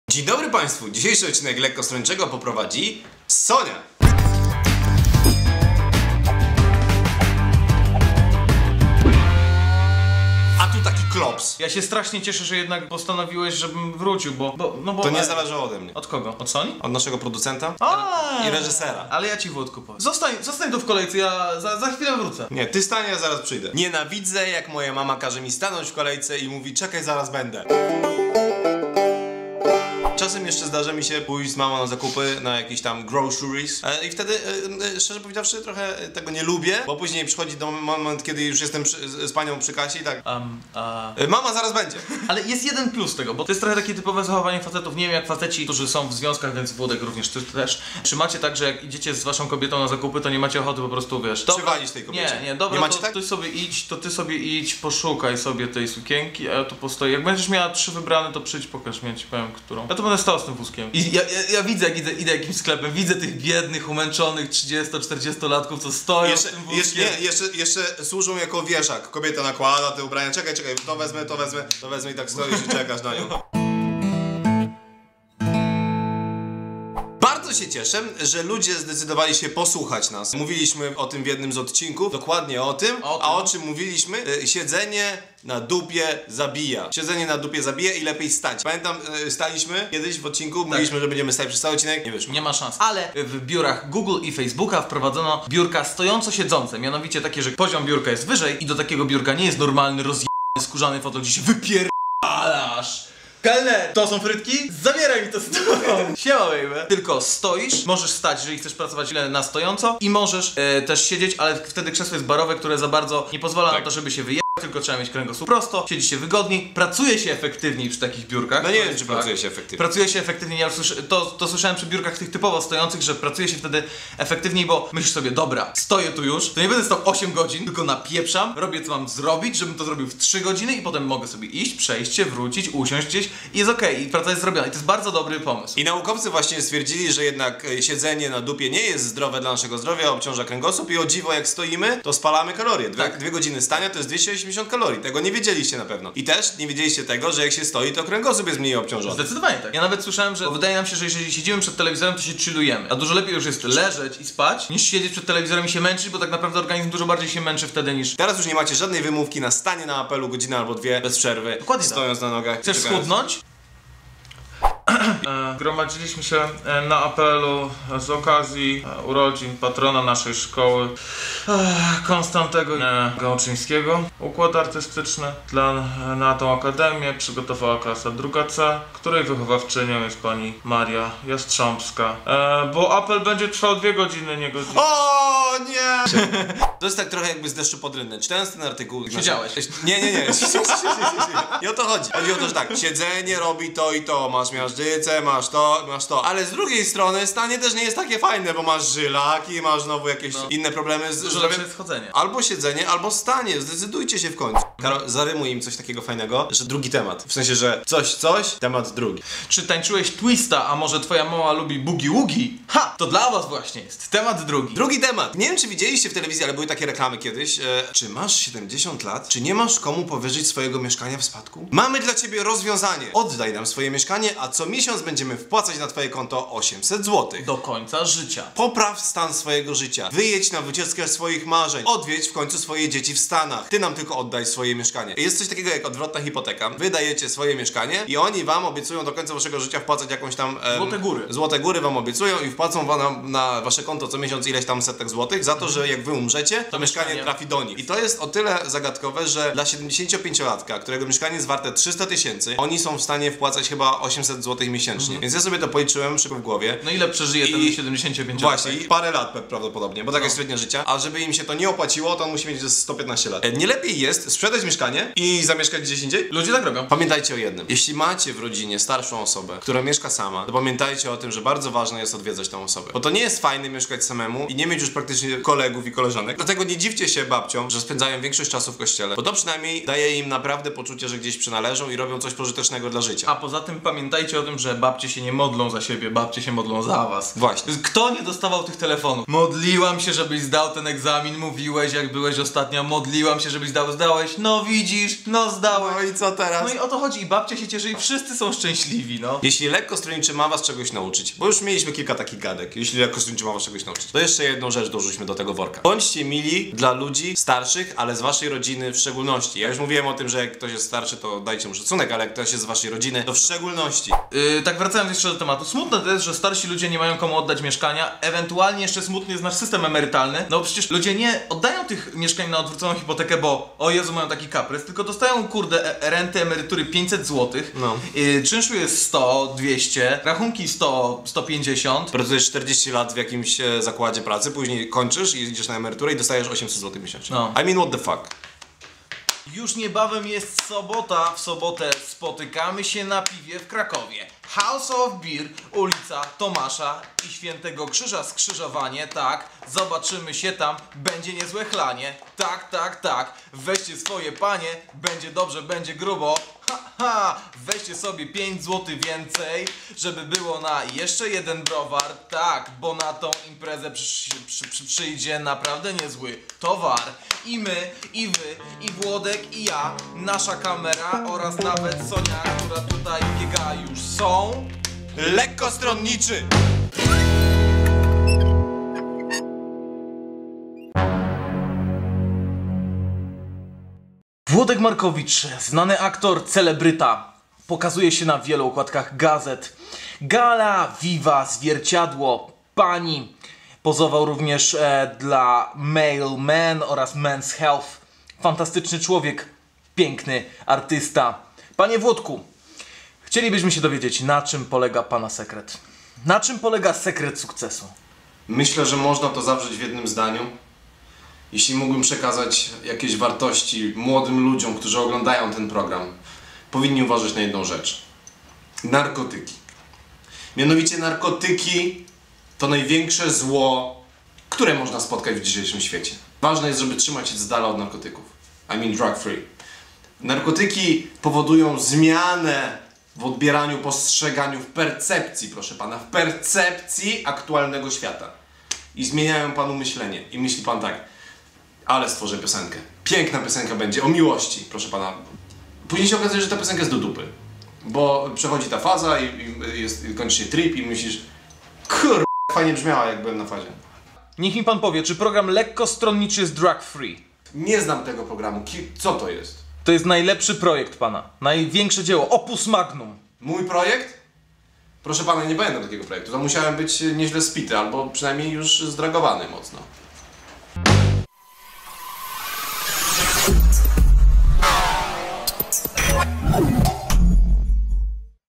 Lepiej stać - LS #142
Stać jest lepiej, niż siedzieć, choć biegać lepiej niż stać. O rodzinie i starych ludziach. O Włodku w Polsce Dokumentalnej.